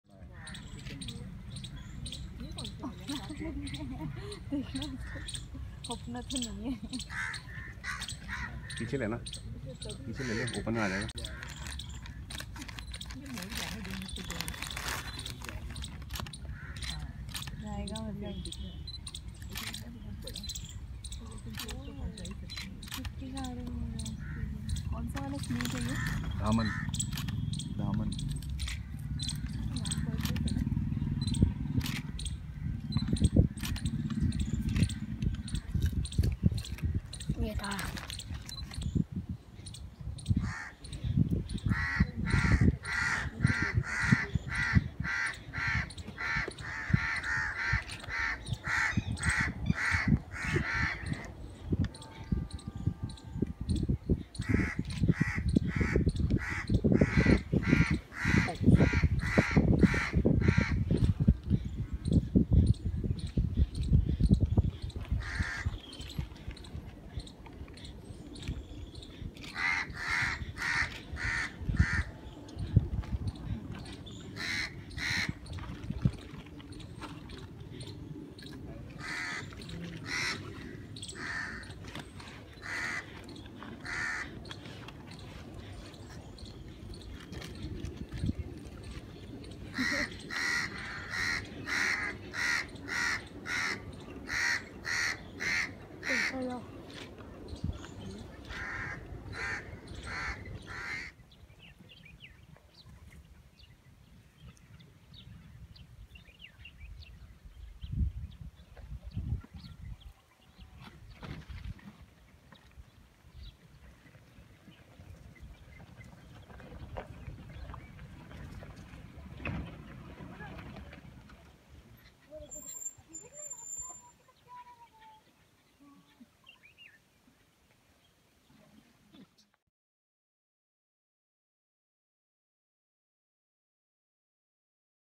Curious peafowls hover around a snake
A harem of peahens with a couple of peacocks is tailing an unidentified snake. They come in close to take a look at the snake and walk with it.

Peafowl is the national bird of India known for its iridescent colourful tail feathers. A male does a mating display by showing off its colourful tail feathers and strutting back and forth in front of the peahens.